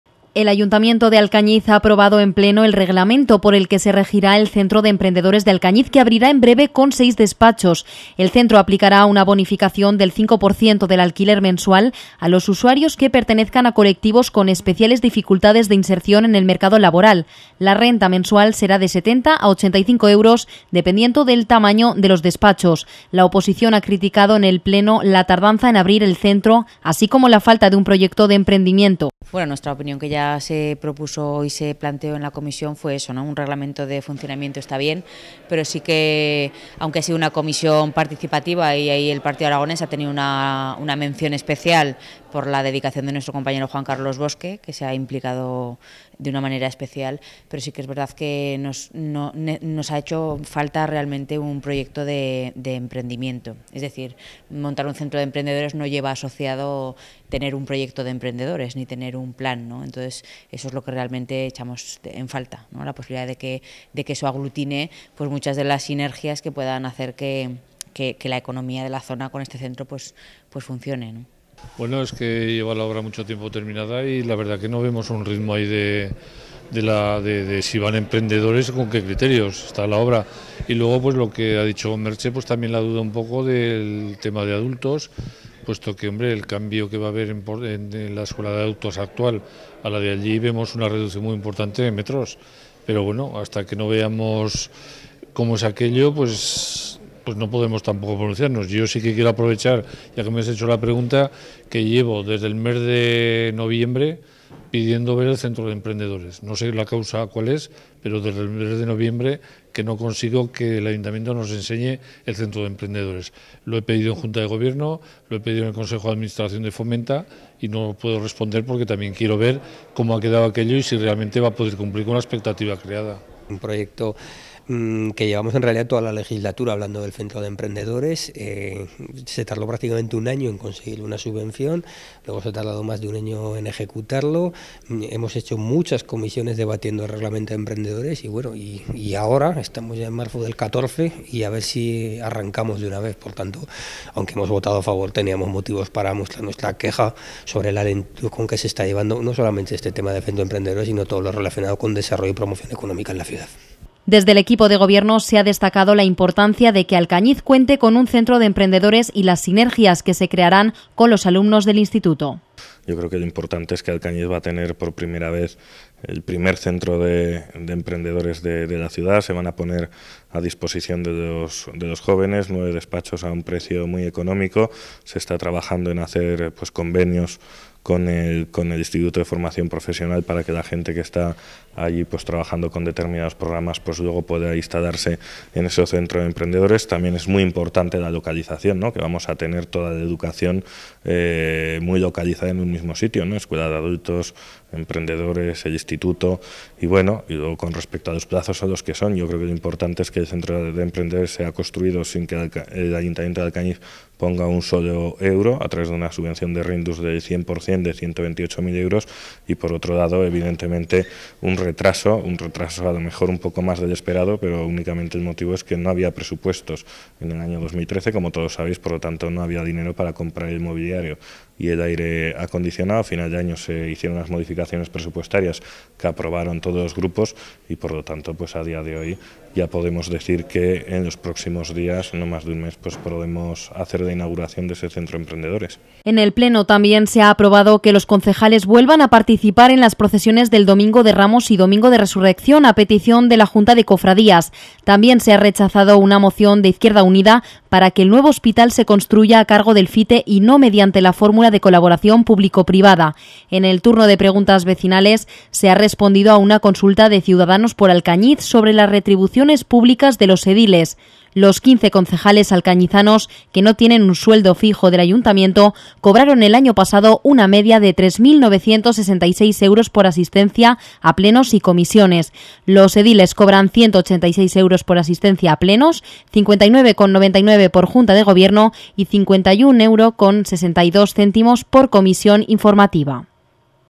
La Comarca.tv - Reglamento Centro Emprendedores de Alcañiz
El Ayuntamiento de Alcañiz ha aprobado en pleno el reglamento por el que se regirá el Centro de Emprendedores de la Ciudad, que abrirá en breve con seis despachos. La sesión se celebró el lunes 3 de marzo y así lo valoraron los portavoces de los grupos políticos.